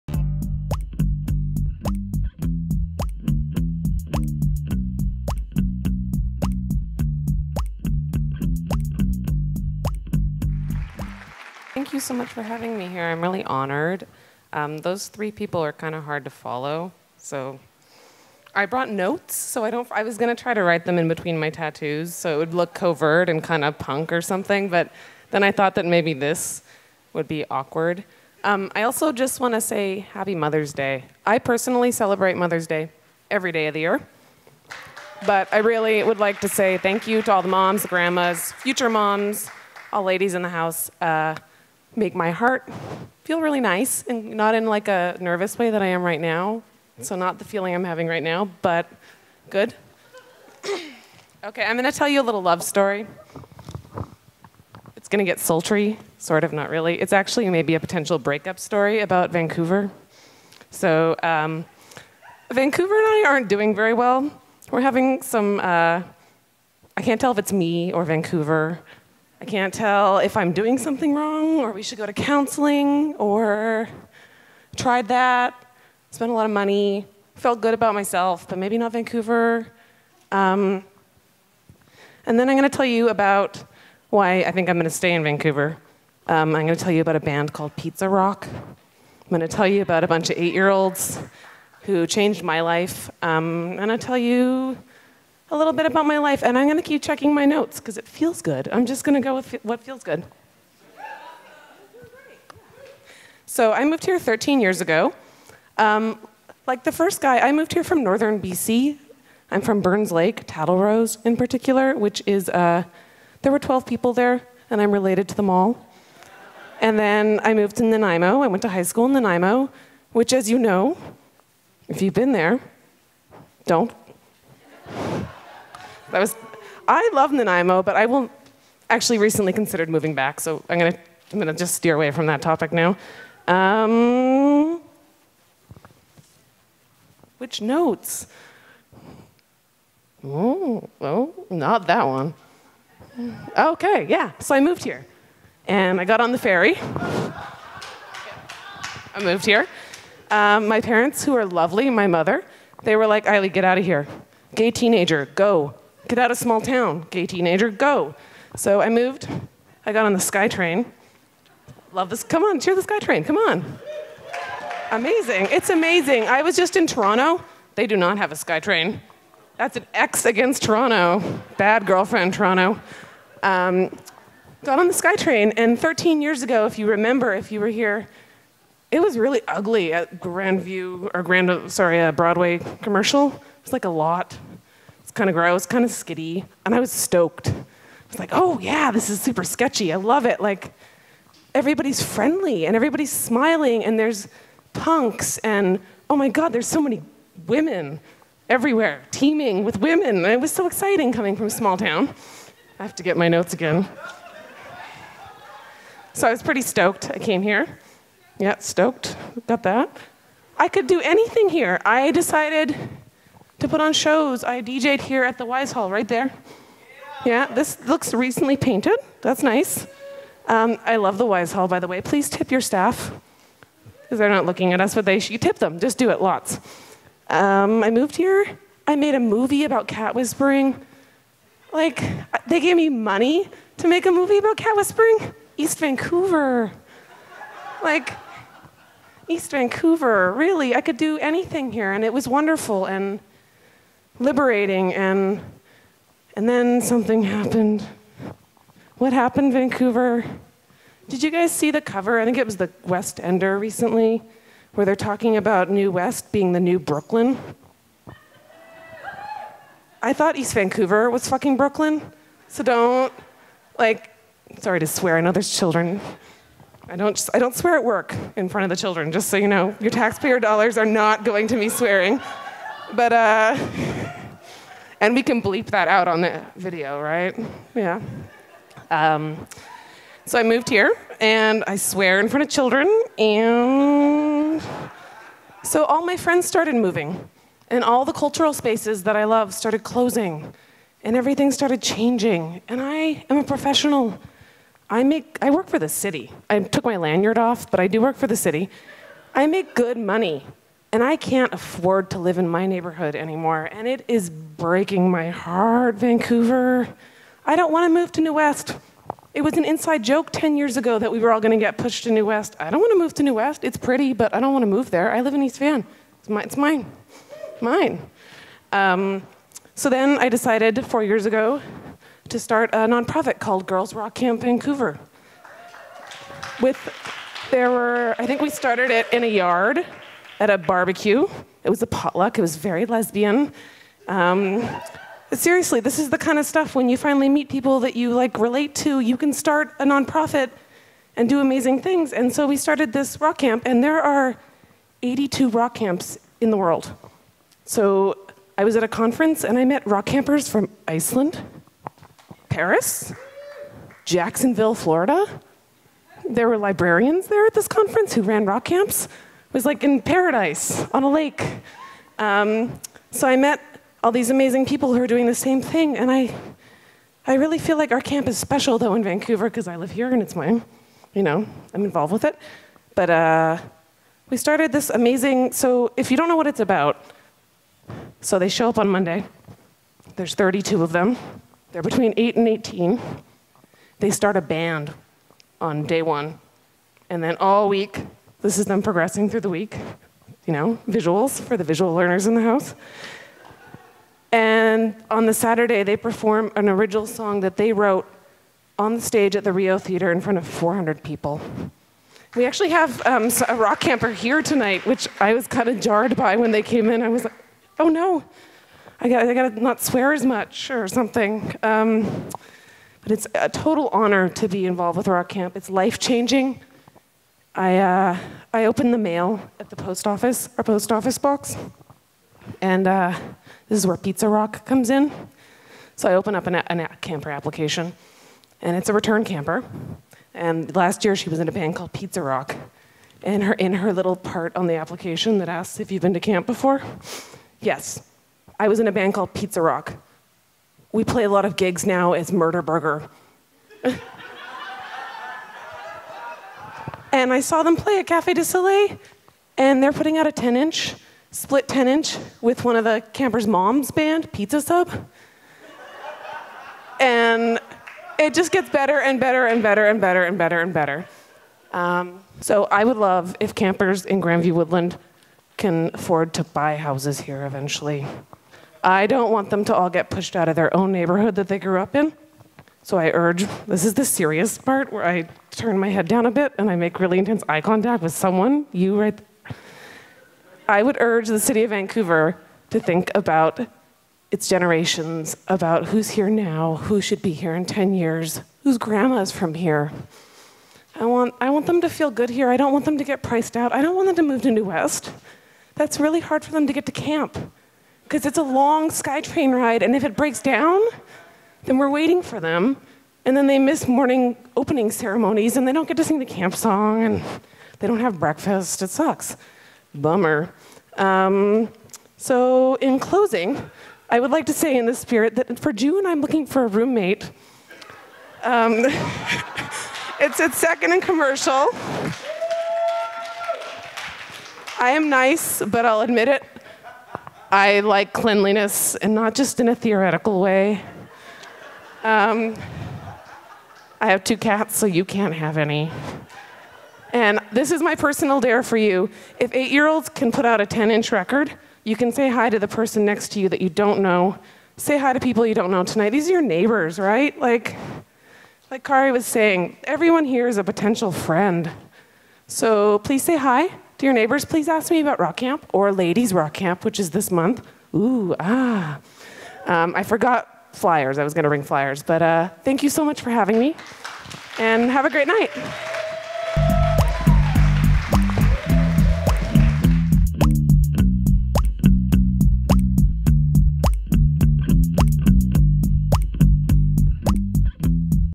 There's No Place Like Home: Aili Meutzner's Grandview-Woodland Story
To launch the Grandview-Woodland Community Planning process, on May 11, 2012 the City of Vancouver held a neighbourhood storytelling event around the theme of home. During this event, local residents shared their personal stories that contributed to the community planning process and the work of the Mayor's Task Force on Housing Affordability. Stories provided an opportunity to hear directly from those for whom we are trying to find housing solutions for and to invite the public into an open ideas competition - re:THINK HOUSING.

In this video, Aili Meutzner shares her story about living in Grandview-Woodland.

Find out more about the Mayor's Task Force for Housing Affordability